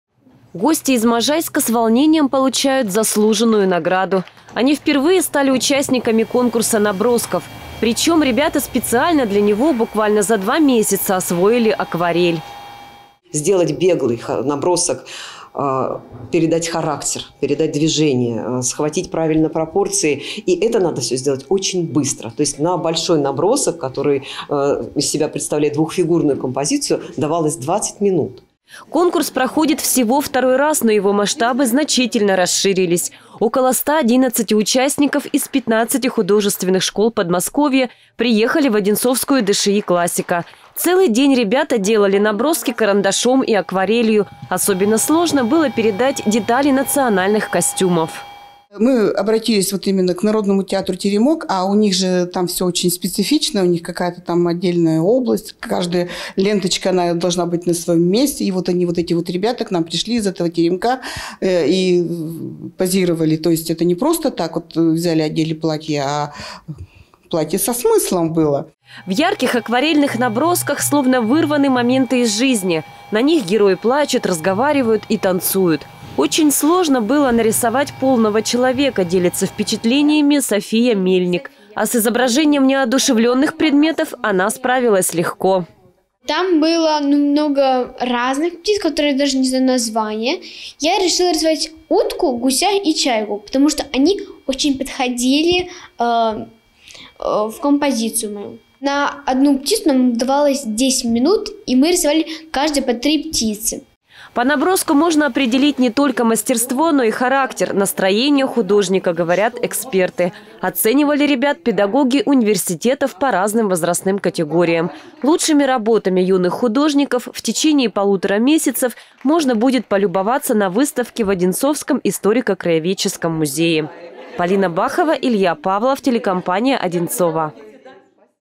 Выставка и конкурс набросков в краеведческом музее
В Одинцовском историко-краеведческом музее состоялось награждение  участников II конкурса набросков. В этот раз в нем приняли участие ребята не только из Одинцовского округа, но и со всего Подмосковья. На выставке картин в музее побывала съемочная группа ОТВ.